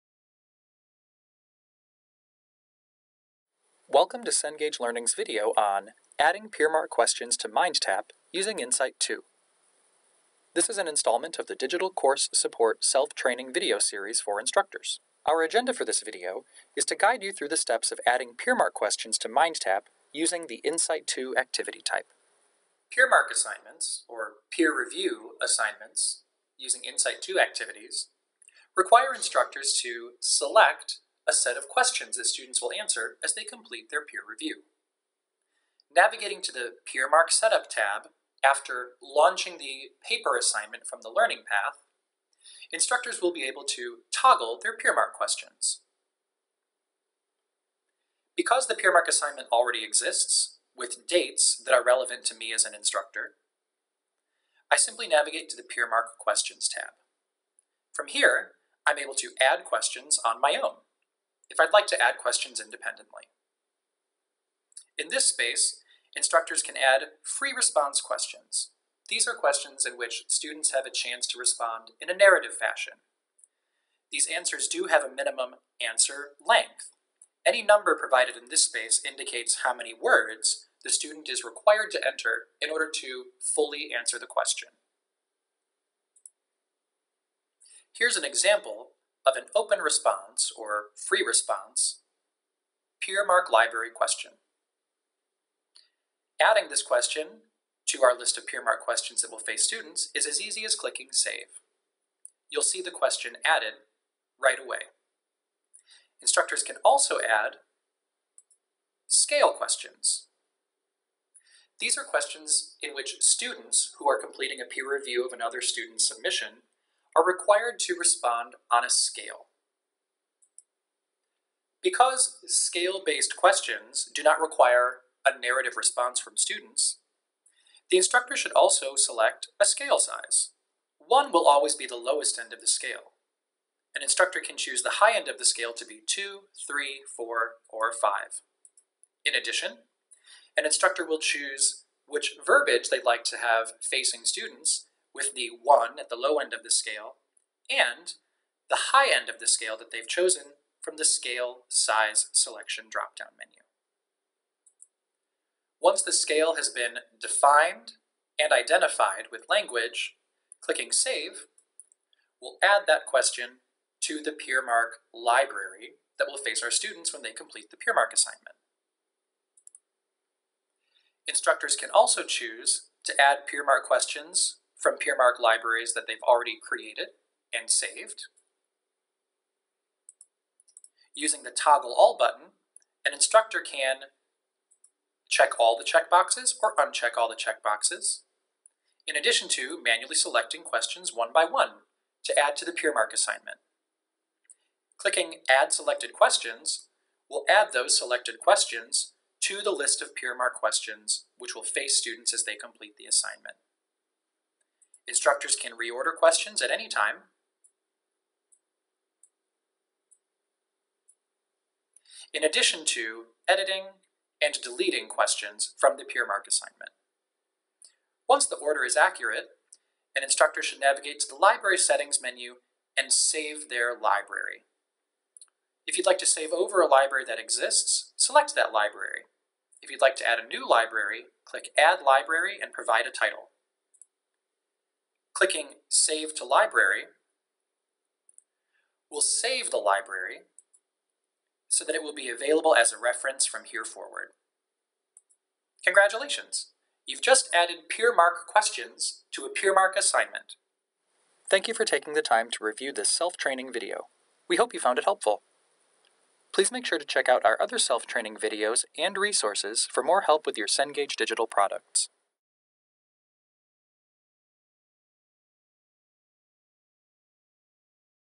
InSite2: Adding PeerMark Questions
This tutorial guides users through the steps of adding peermark questions to MindTap using the InSite2 activity type.